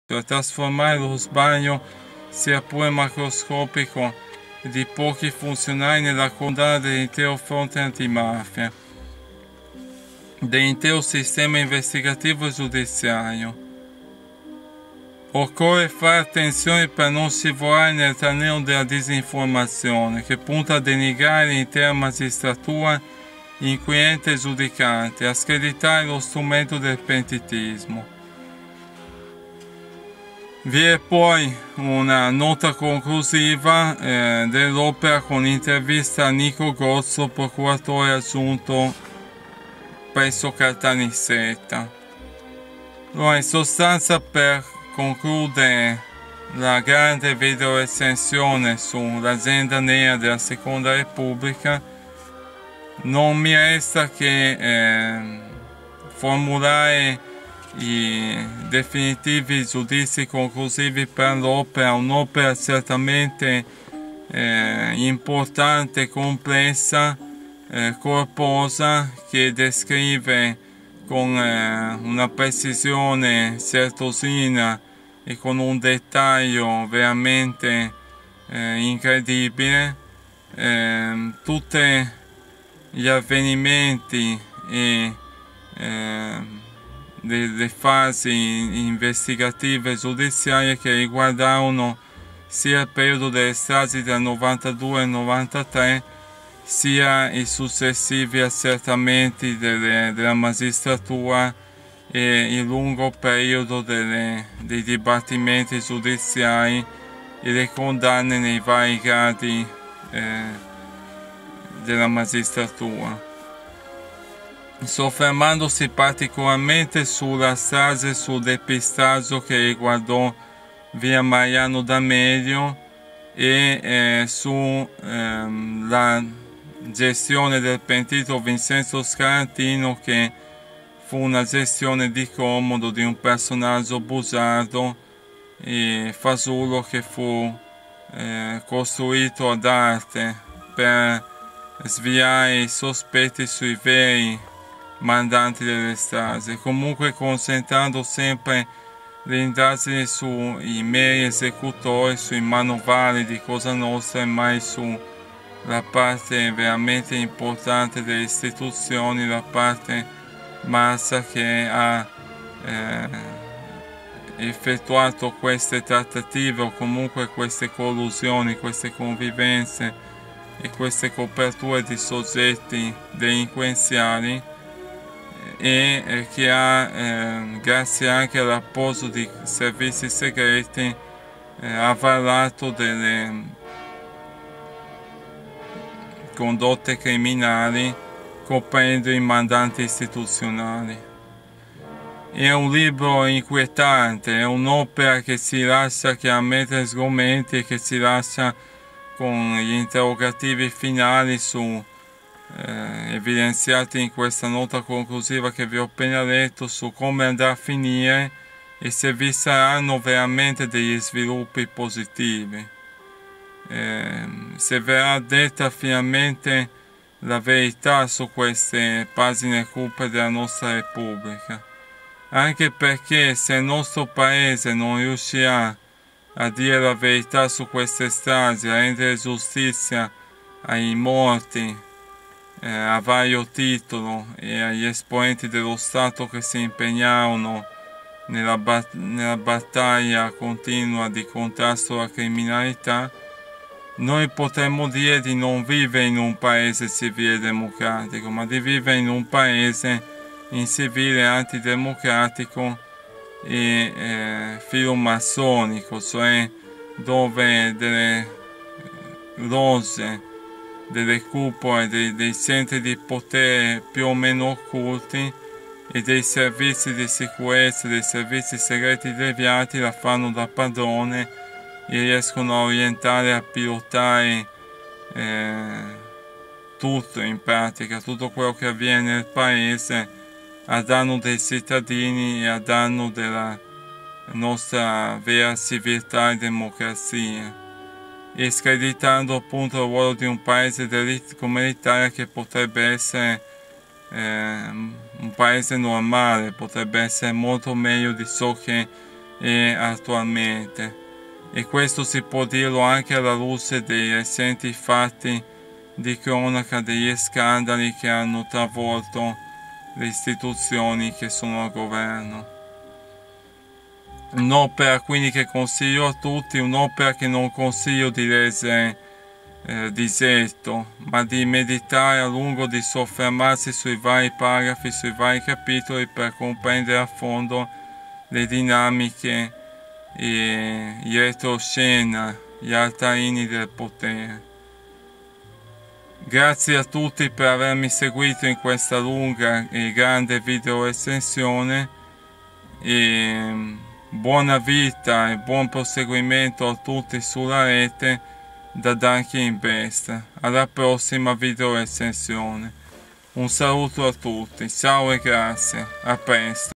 Grande VideoRecensione -- L'Agenda Nera della Seconda Repubblica 34/34
Titolo: L'Agenda Nera della Seconda Repubblica
Sottotitolo: Via D'Amelio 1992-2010. Un depistaggio di Stato.
Autori: Giuseppe Lo Bianco, Sandra Rizza
Editore: Chiarelettere

Descrizione

Illuso chi pensa che lo Stato possa processare se stesso.
Leonardo Sciascia, scrittore e saggista.
Il compito di un dottore è guarire i pazienti, il compito di un cantante è cantare. L'unico dovere di un giornalista è scrivere quello che vede. Anna Stepanovna Politkovskaja, giornalista.
L'instabilità e la mancanza di credibilità istituzionale oggi è tale che potrebbe produrre tentazioni stragiste. Nico Gozzo, procuratore aggiunto a Caltanissetta.
Questa è la storia del depistaggio che ha coperto i mandanti di via Mariano D'Amelio, offrendo all'opinione pubblica un falso pentito e una falsa verità sulla strage. E' la storia del depistaggio che ha portato alla sbarra, utilizzando pressioni e minacce, tre balordi di borgata con l'accusa di essere i manovali della strage, e poi un pezzo della Cupola di Cosa Nostra, sulla base di un'indagine della polizia. Ed è proprio da quest'indagine che oggi, sotto i colpi delle nuove rivelazioni del pentito Gaspare Spatuzza, si sta sgretolando con il rischio di incrinare la credibilità dello Stato, in una fase delicatissima della lotta alla mafia.
La verità su via Mariano D'Amelio è ancora lontana perché è stata insabbiata, un falso pentito ha retto ben sei processi e due sentenze della Suprema Corte. Perché? Com'è possibile che investigatori considerati pilastri dell'antimafia abbiano dato credito ad un'assurdità così clamorosa? Lo Bianco e Rizza ce lo raccontano fotografando questi ultimi 18 anni di complici mistificazioni.
L'AGENDA NERA si apre sul teatro della strage, ne ricostruisce i retroscena raccontandone, per la prima volta, le indagini, i processi, la fiera di dichiarazioni, ritrattazioni, arresti e minacce. E il ribaltone politico che ha portato alla nascita di Forza Italia. Fino alla nuova inchiesta di Caltanissetta che vede indagati alti ufficiali di polizia.
In attesa degli esiti dell'indagine, una domanda che nasconde verità scomode si impone: agirono da soli? Dietro la falsa pista Scarantino, secondo i pm potrebbe celarsi un progetto eversivo per nascondere i mandanti occulti della strage. La verità la chiedono i familiari e i cittadini che in Borsellino hanno visto il simbolo più alto delle istituzioni. Quelle stesse che probabilmente lo hanno tradito.
Chiude il libro un'intervista al procuratore aggiunto di Caltanissetta Nico Gozzo.
Si informa il pubblico di videospettatori che è stata dedicata la massima attenzione possibile a quest'opera data la portata fondamentale per capire la storia e i meccanismi occulti del nostro Stato, con più di 330 minuti di sintesi ed analisi, senza dubbio la realizzazione più imponente mai realizzata prima da alcun volontario singolo senza fondi esterni di alcun tipo.

Buona visione della VideoRecensione e buona lettura dell'opera.
In fede
Dankingbest